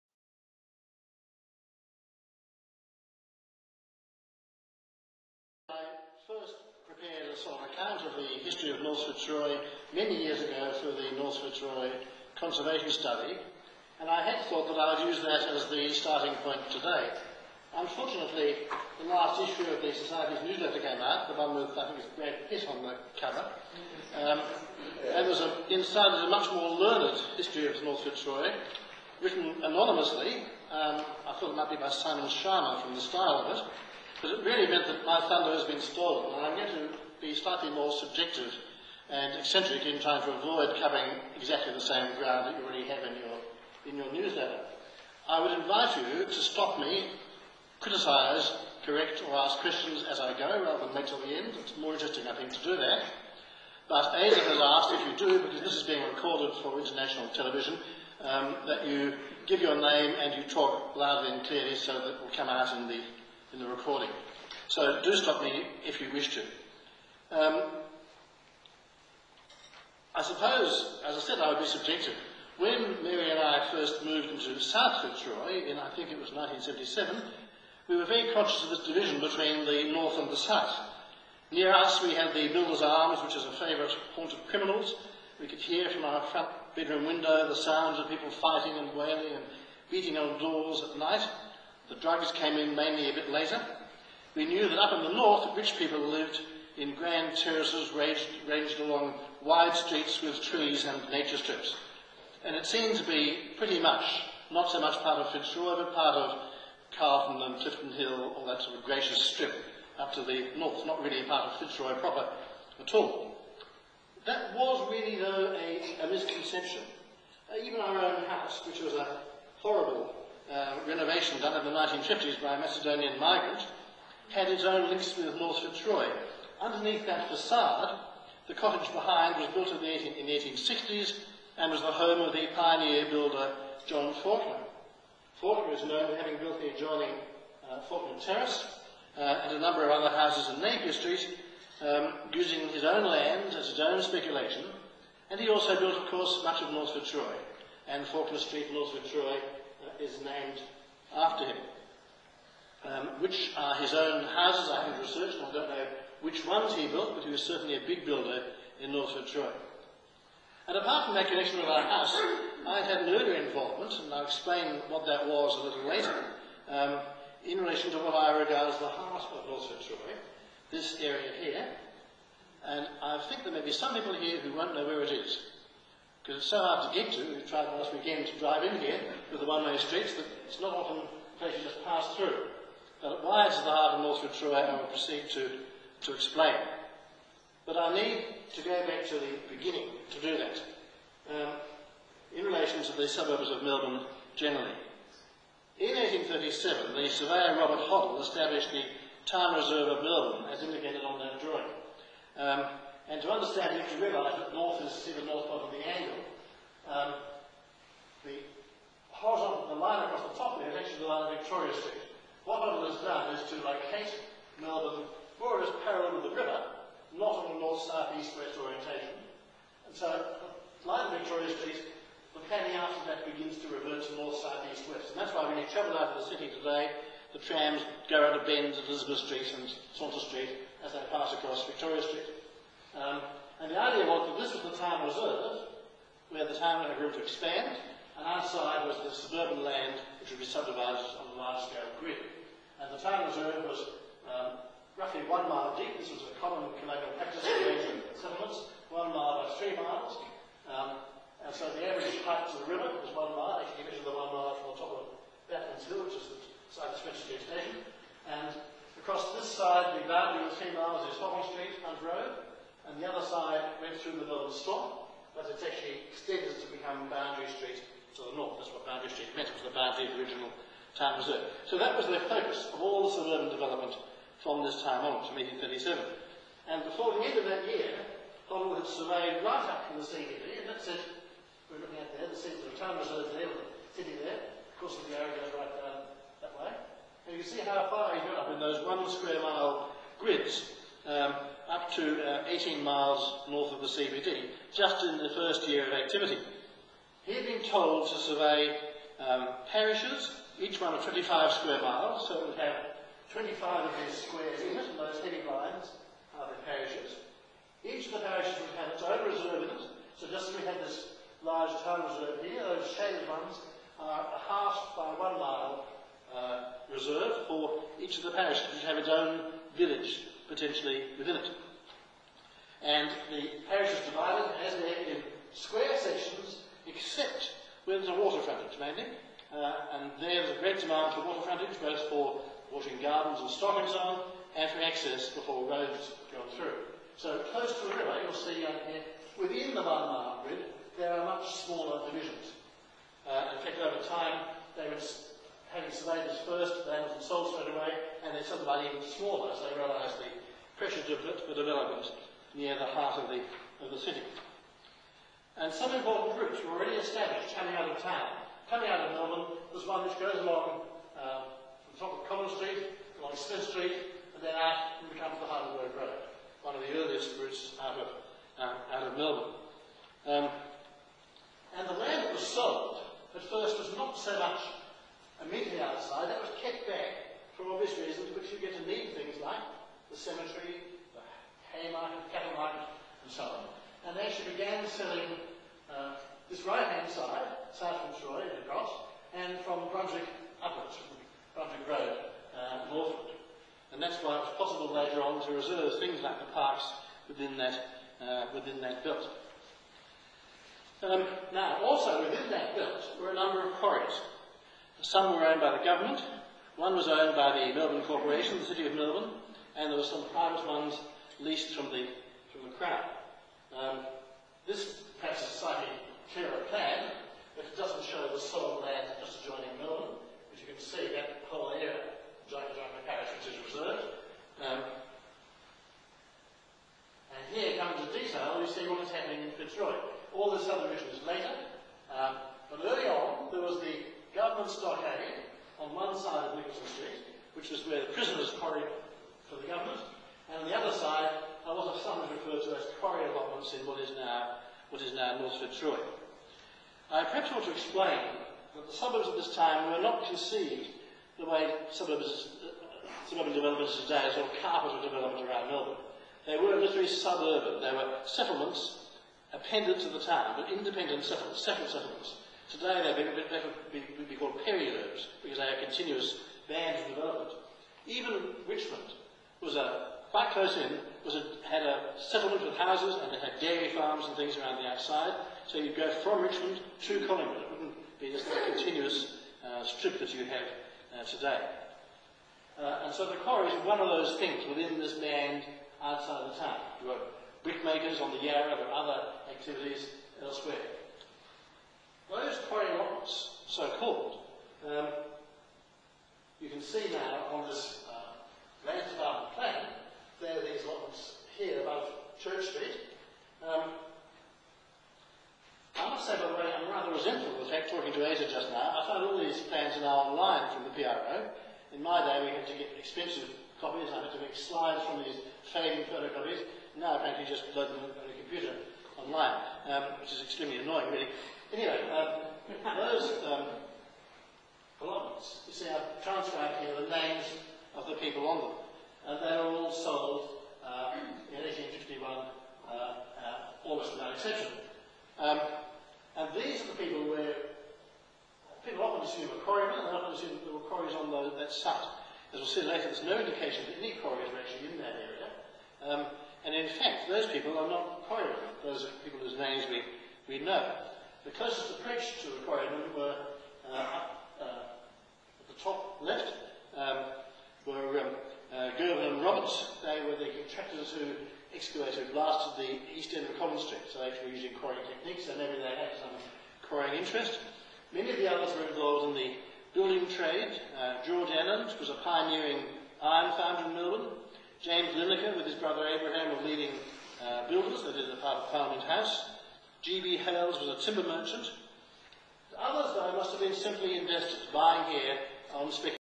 Fitzroy's Northern Renaissance: Part 1
Miles Lewis, architectural historian and Professor in the Faculty of Architecture, Building and Planning at University of Melbourne and founding member of the Fitzroy History Society speaks about the creation of North Fitzroy and its relation to South Fitzroy and the inner suburbs generally.